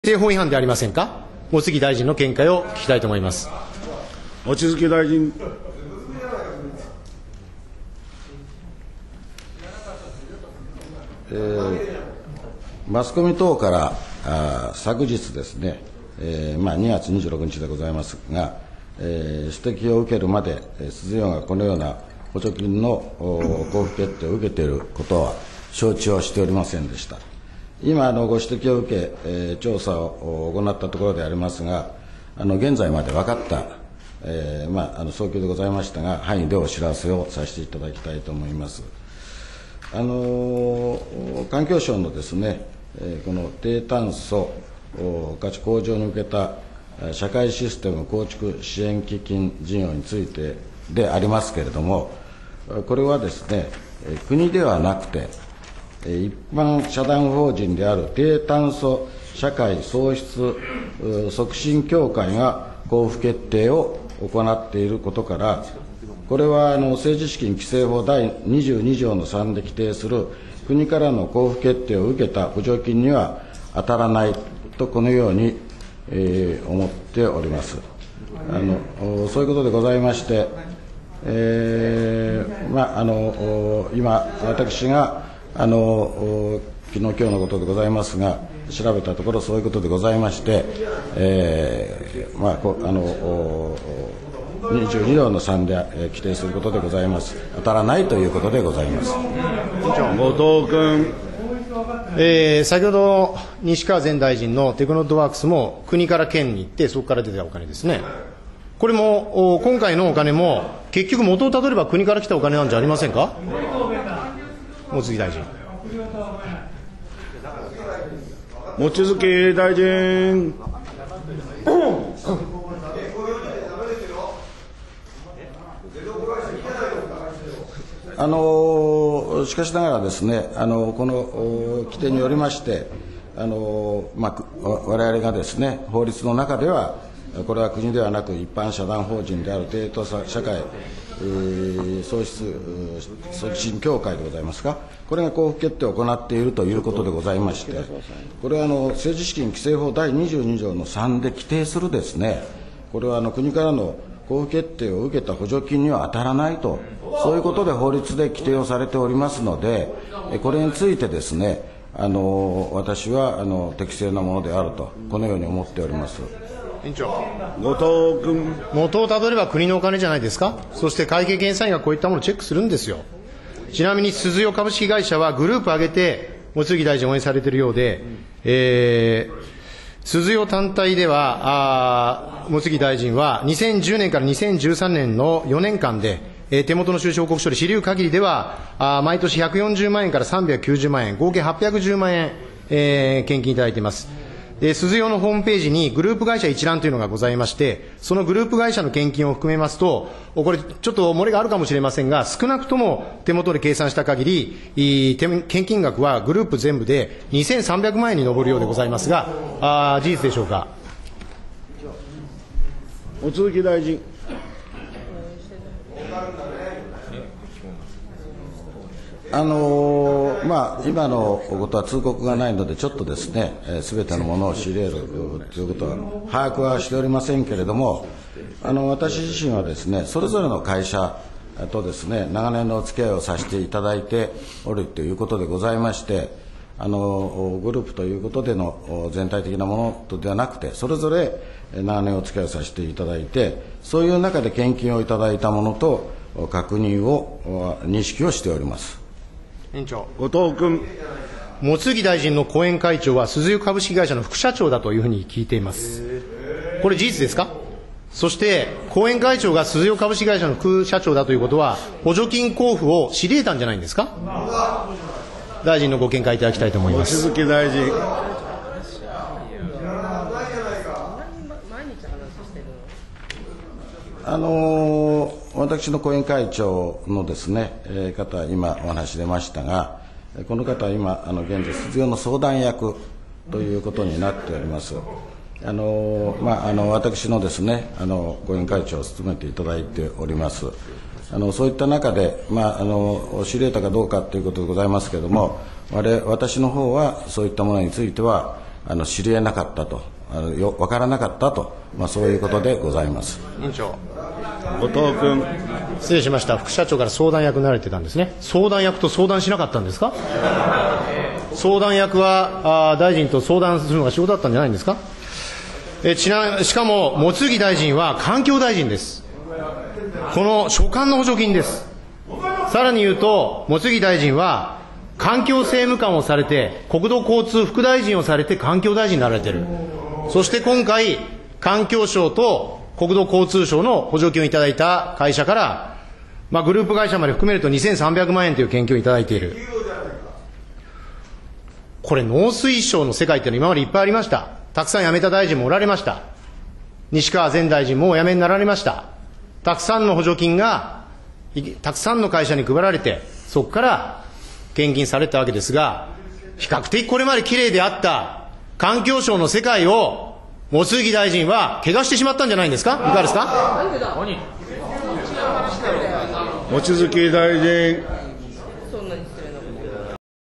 望月環境相 補助金交付企業から献金2/27 衆院
望月義夫環境相　2/27 衆院予算委員会　後藤祐一（民主党）の質疑